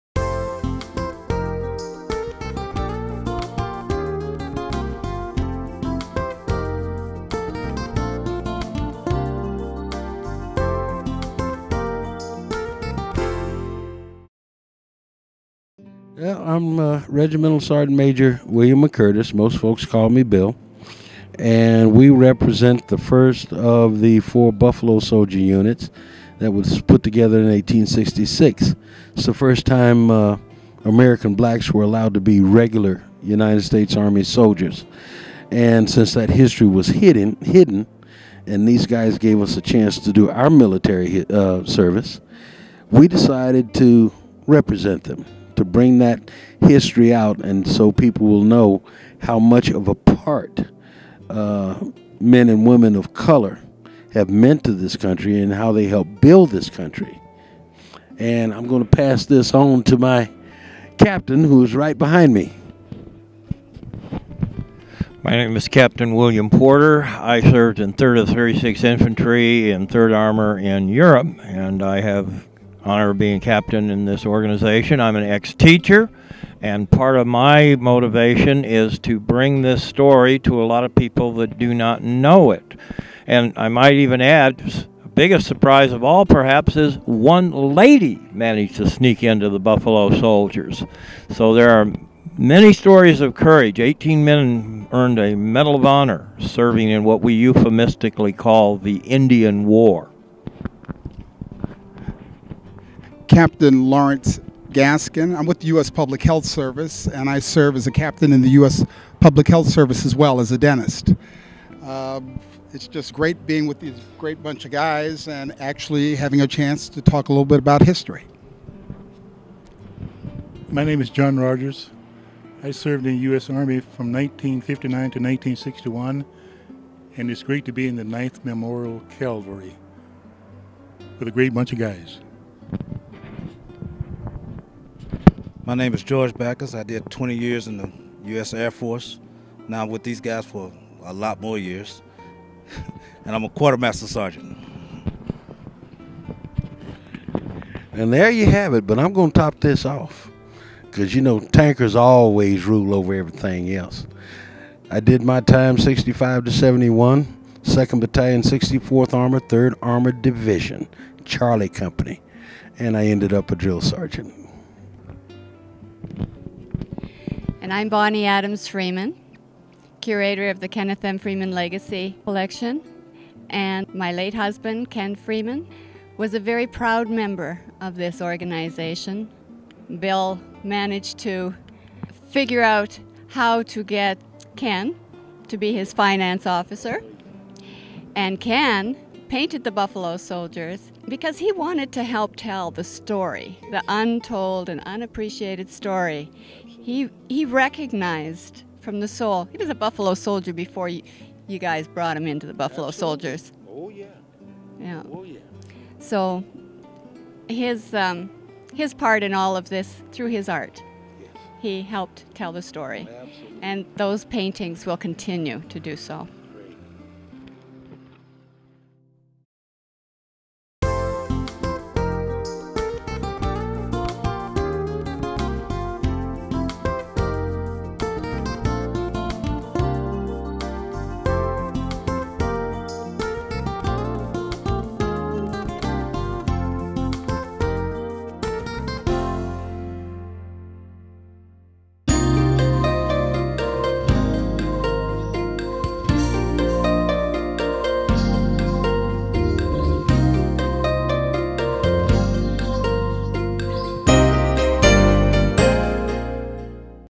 The Buffalo Soldiers Remember Kenneth M. Freeman
Buffalo soldiers were sent to the remote areas of the Old West.  Many of their outposts are now major Southwestern cities.  This video features several Buffalo Soldiers dressed in their regalia at the 20th National Festival of the West 2010.  Artist Kenneth M. Freeman was a Buffalo Soldier in the 9th Memorial Calvary in Arizona and Ken painted the Buffalo Soldiers.